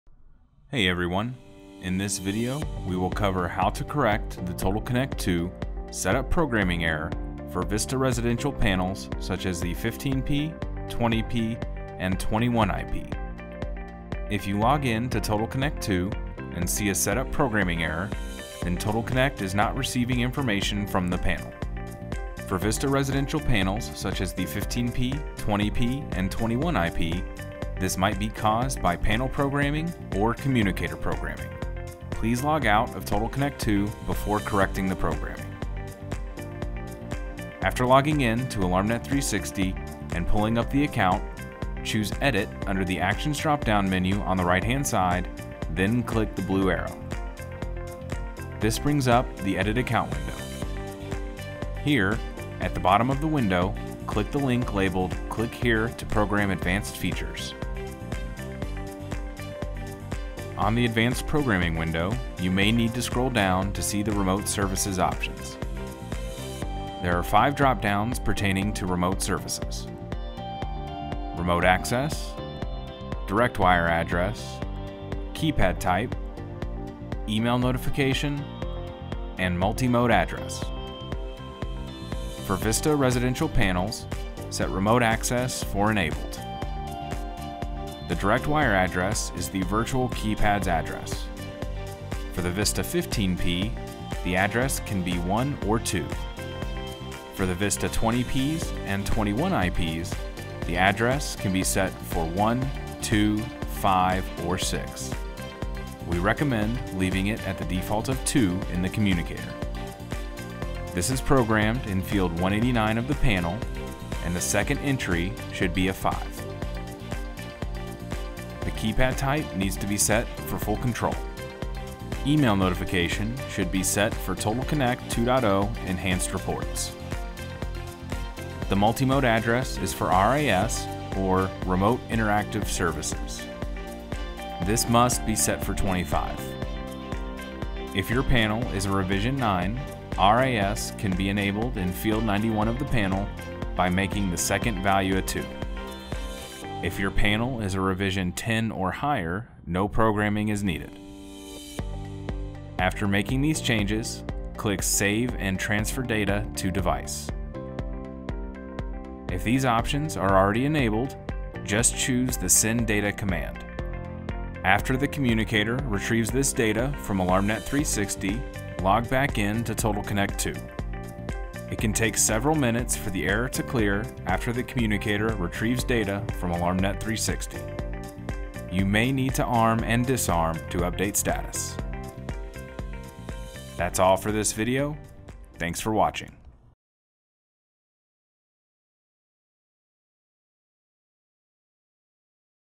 How to correct the Total Connect 2.0 setup/programming error for VISTA Residential Panels - Resideo
for more information on Resideo Security products.

This video covers how to correct the Total Connect 2.0 setup/programming error for VISTA residential panels such as the 15P, 20P, or 21IP.